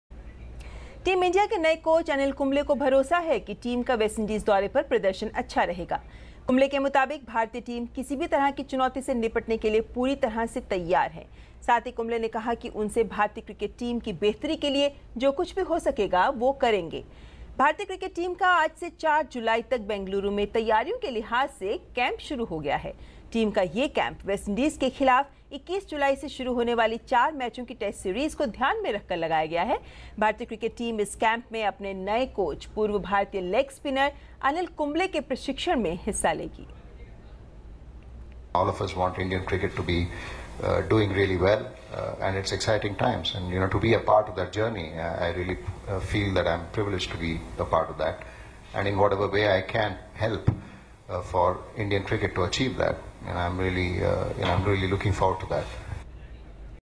Team India is ready for every challenge: Anil Kumble
The newly elected head coach of Indian Cricket Team Anil Kumble has said that Team India is all set to face every challenge.